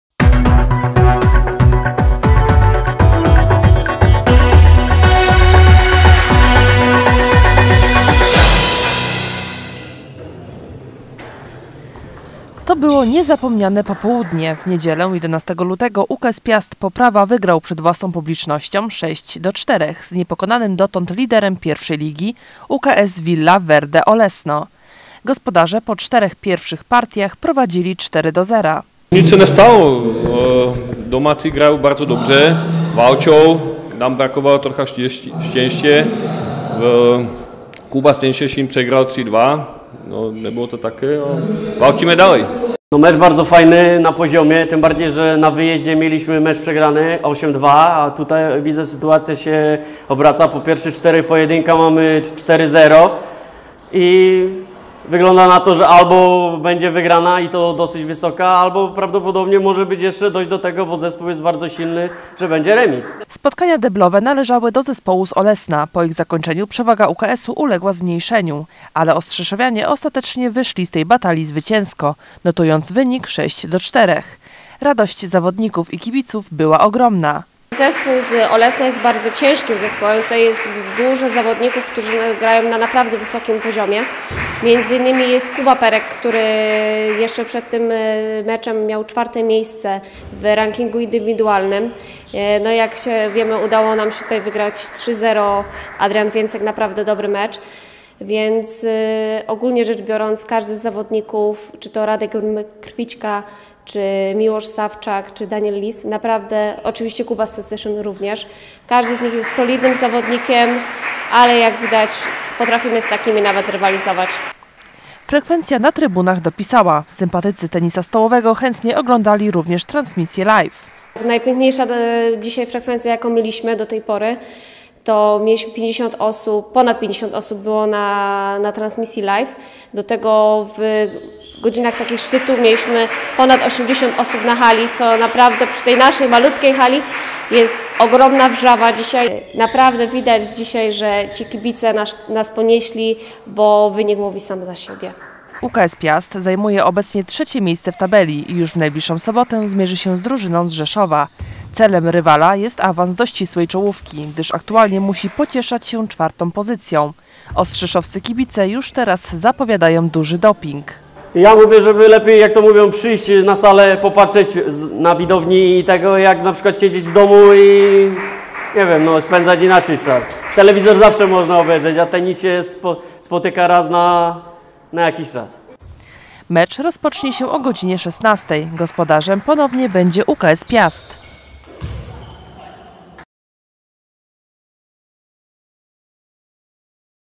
UKS Piast Poprawa pokonał lidera
To było niezapomniane popołudnie. W niedzielę (11 lutego) UKS Piast Poprawa wygrał przed własną publicznością 6:4 z niepokonanym dotąd liderem pierwszej ligi - UKS Villa Verde Olesno.